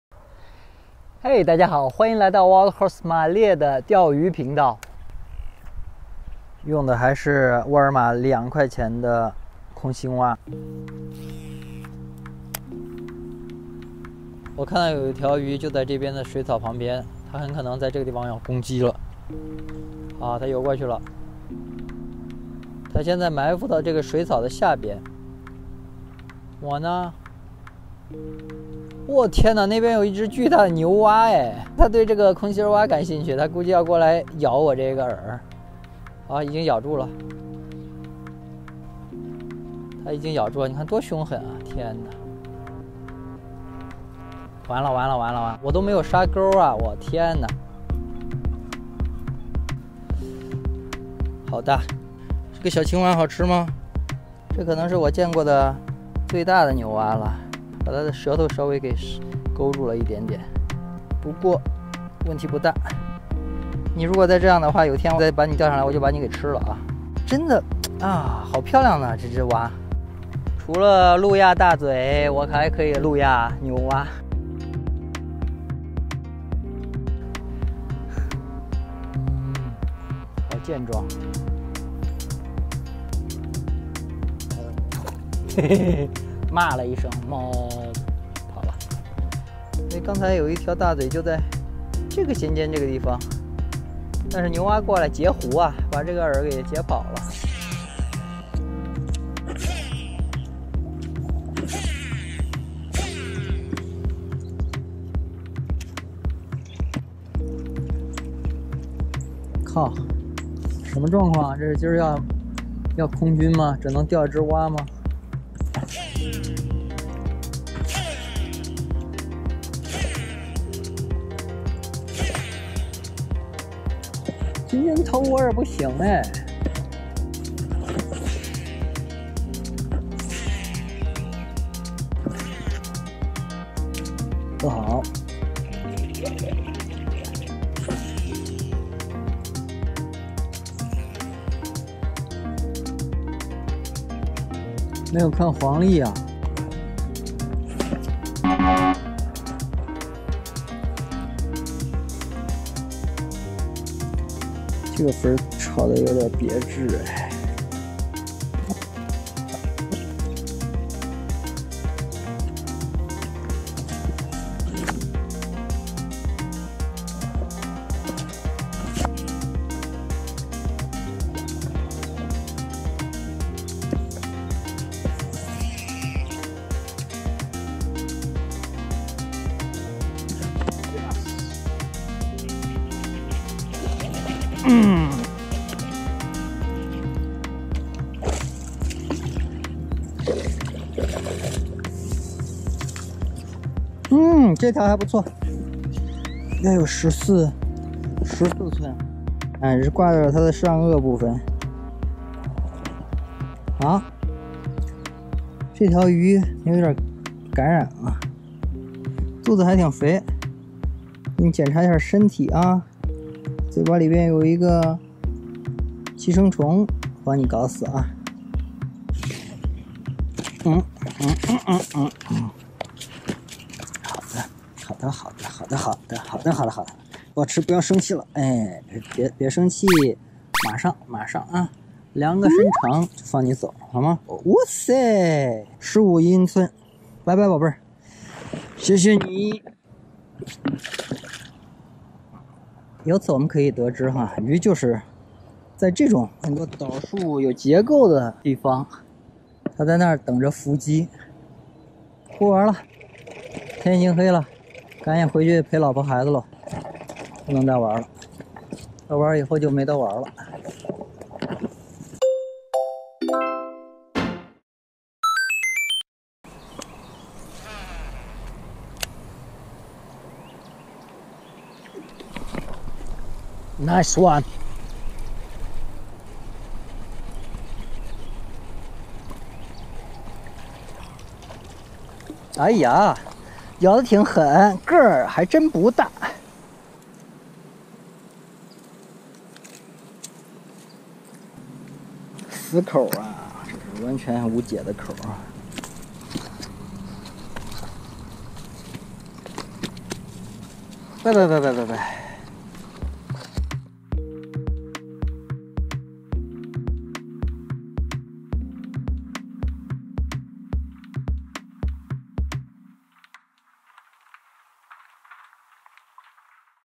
「路亚路遇」等着大嘴鲈伏击水面假饵，却杀出这个家伙来截胡！｜便宜假饵挑战
还是那个2刀的空心蛙，表现一直不错，这天傍晚却路上来这么个家伙. 相煎何太急呀